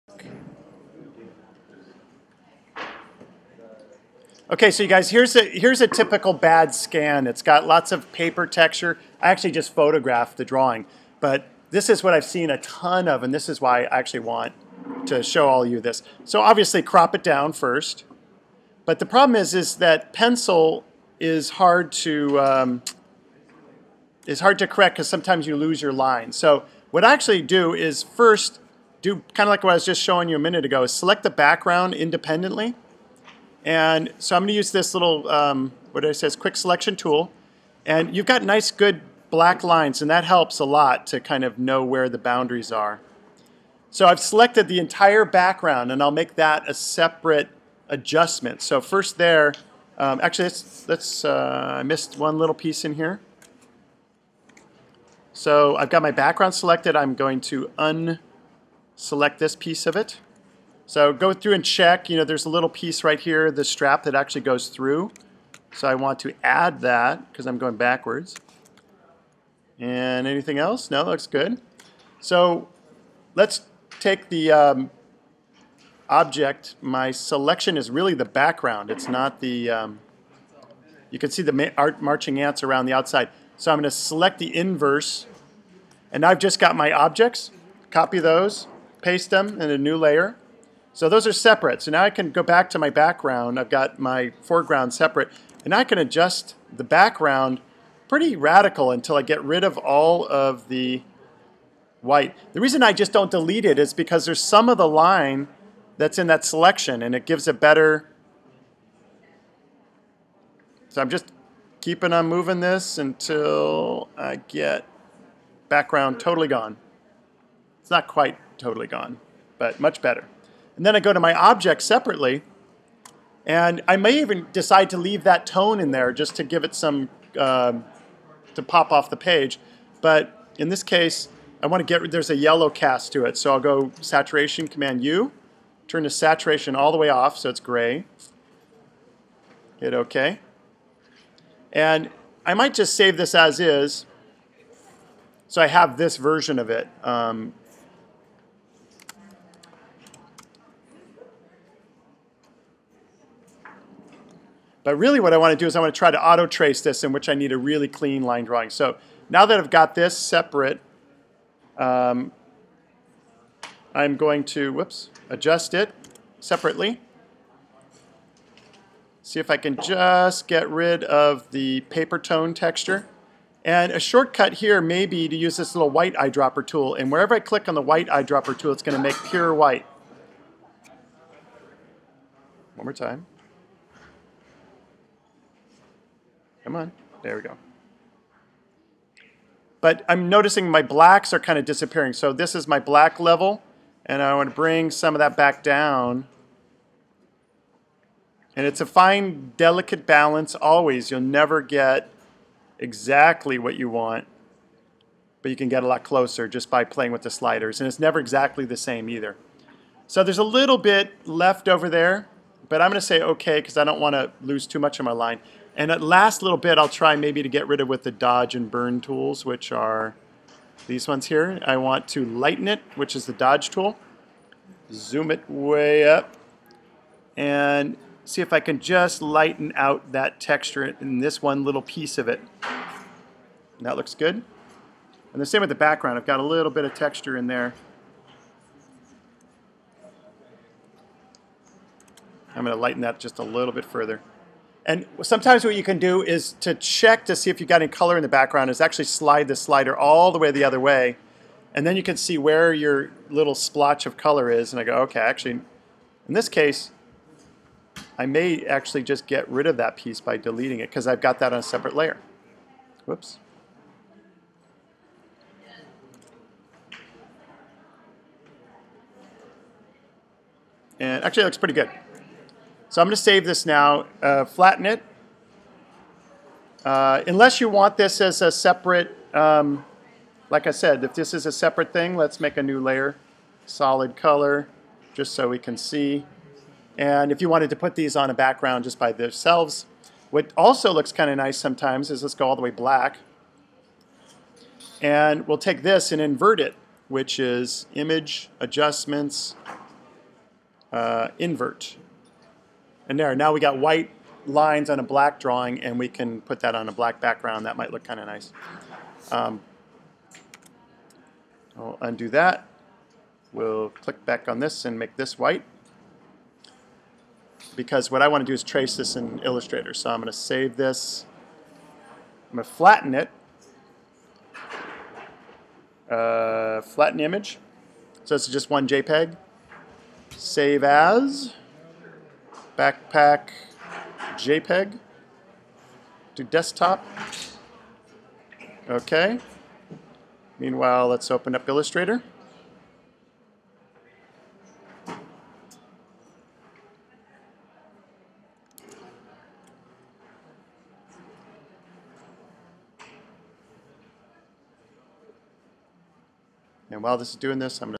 clean up a bad scan
tutorial on settings for adjusting a scanned line drawing